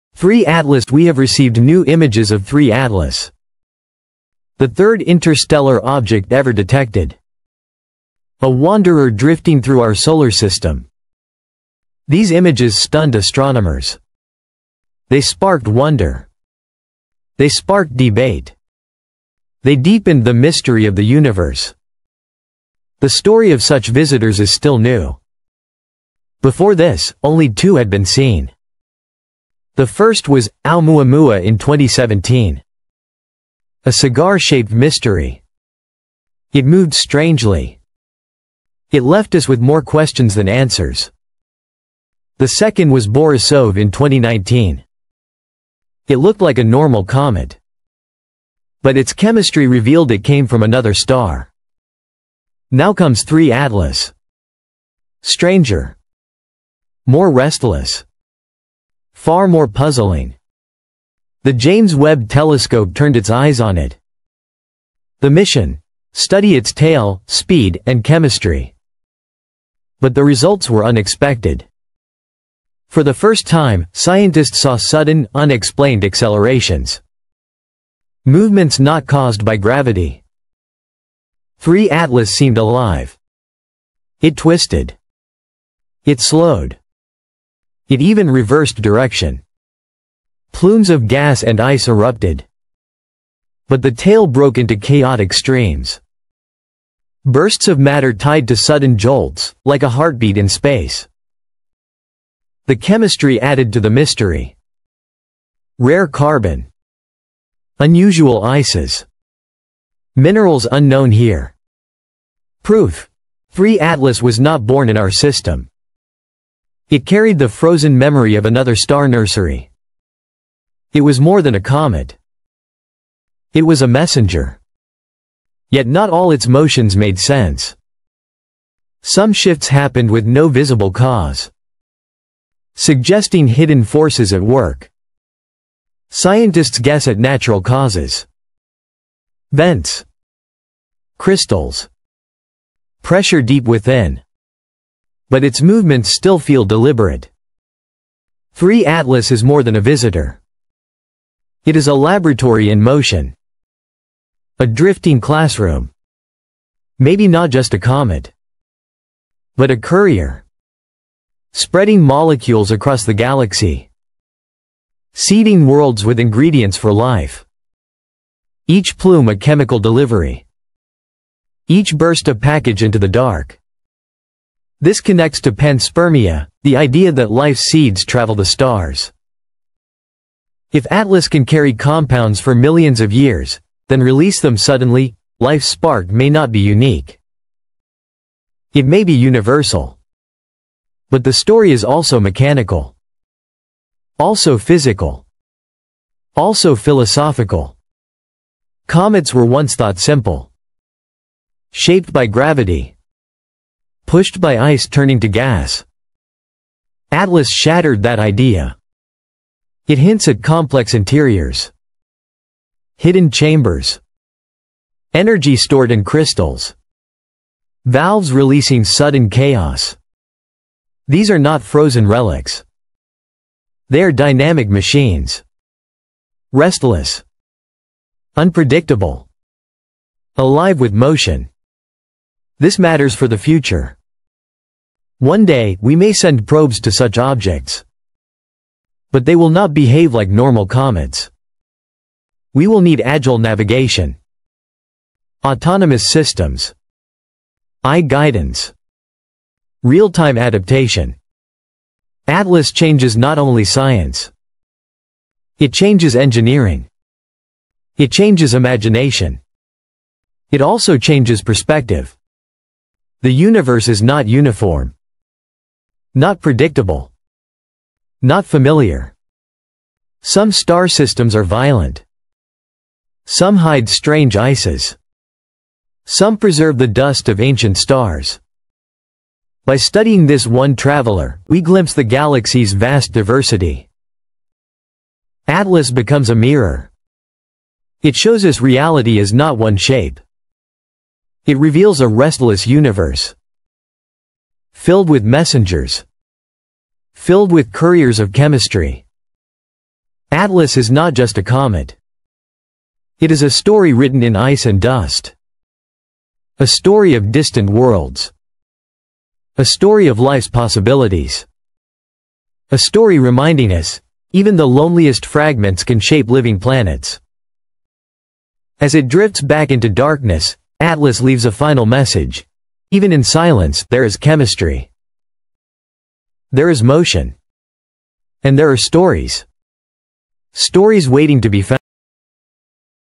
Strange Acceleration Patterns Spotted Near 3I/ATLAS… James Webb Infrared Imaging Reveals All
The James Webb Space Telescope has once again astounded the scientific community, this time with a discovery that could totally change our understanding of interstellar visitation.  Using cutting-edge infrared imaging, Webb discovered weird and unexpected acceleration patterns near the mysterious cosmic object 3I ATLAS.  What makes this discovery so interesting is that the patterns do not correspond to the conventional gravitational or solar radiation-driven effects expected by astronomers, igniting new discussions over the true nature of these cosmic wanderers.

 In this video, we delve deep into the most recent Webb data, deciphering what these acceleration patterns may indicate and why they differ from anything previously seen.  From analogies to other interstellar objects such as Oumuamua and Borisov to speculations ranging from normal cometary outgassing to exotic possibilities, the disclosures concerning 3I ATLAS have sparked both excitement and controversy in the astronomical community.

 Join us as we investigate how Webb's unmatched infrared capabilities are transforming the way we perceive interstellar visitors.  Could 3I ATLAS hold clues to the composition of other star systems?  Are these acceleration patterns indicative of unknown physics, or perhaps something artificial?  The answers may be found in Webb's most recent observations, with far-reaching consequences beyond our solar system.

If you're interested in the James Webb Telescope, interstellar puzzles, and the search for solutions beyond Earth, be sure to watch till the conclusion.